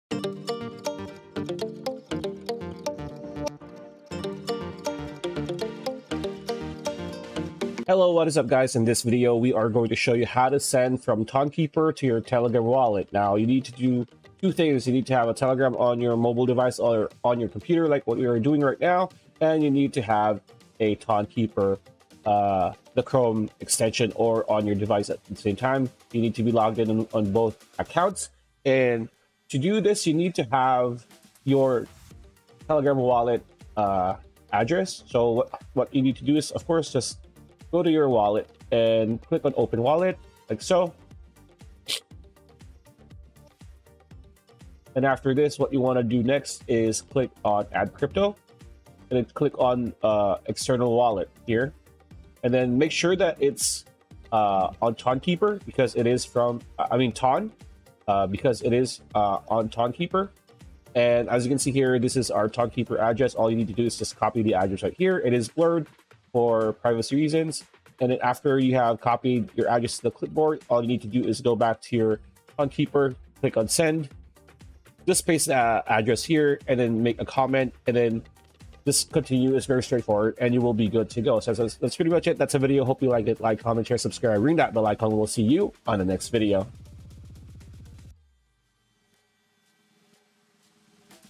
How to Send from Tonkeeper to Telegram Wallet (Best Method)
If you're looking for the best method to send from Tonkeeper to Telegram Wallet, then this video is for you! Learn how to transfer seamlessly between wallets.